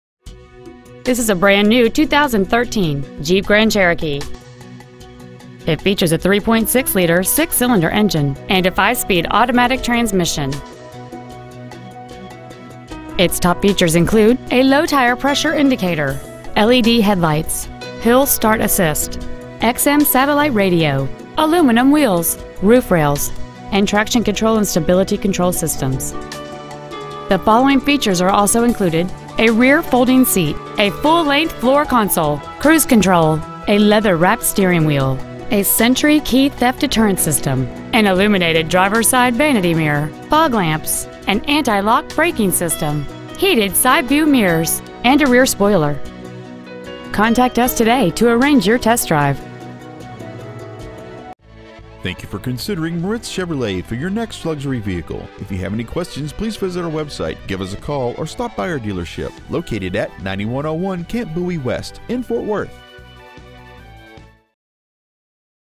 2013 Jeep Grand Cherokee Waco TX
Year : 2013
 Make : Jeep
 Model : Grand Cherokee
 Engine : Gas/Ethanol V6 3.6L/220
 Trans . : Automatic
 Exterior : BLK FRST GRN PL

 Interior : BLK CLTH LOW-BACK BUCKET
 Stock : 637093